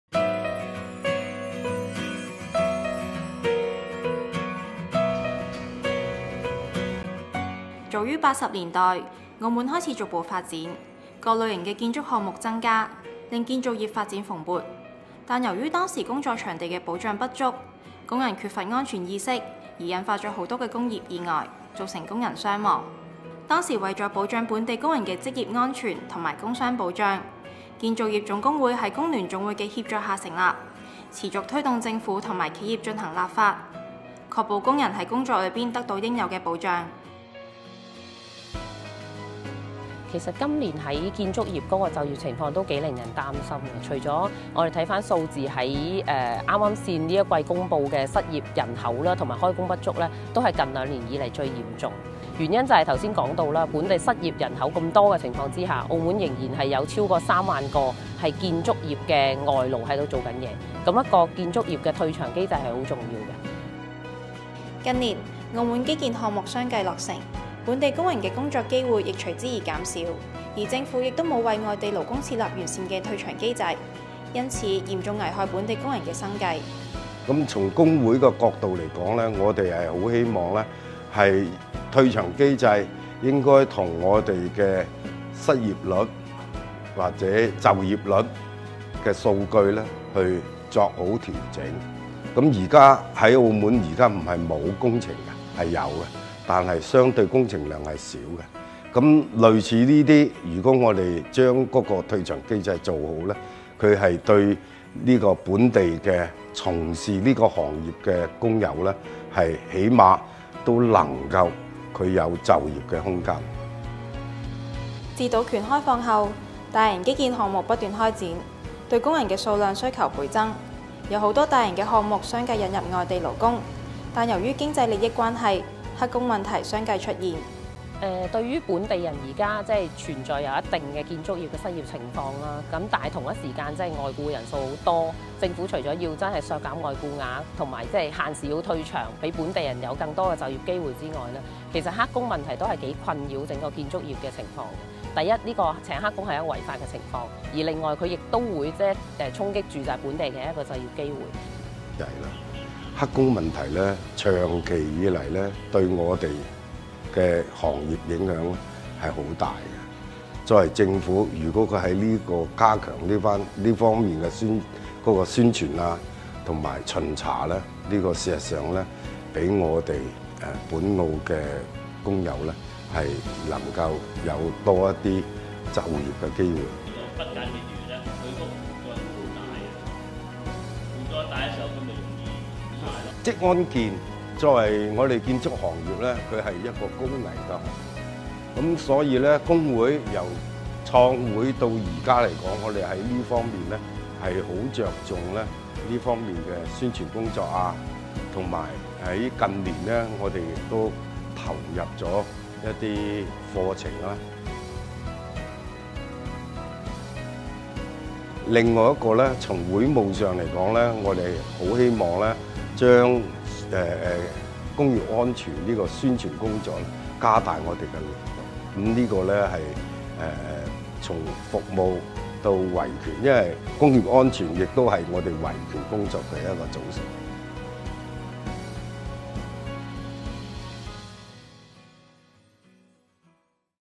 【工聯故事】同心齊撐建築行友
建造業係一項勞動強度大、工作危險性高的行業。加上開工不穩，收入無保證，兼且行內存在大量外勞及黑工；相關法制又不完善，令本地工人無實質的就業保障。
雖然你唔一定做呢行，都花少少時間了解下，其實建築工人為澳門貢獻好大。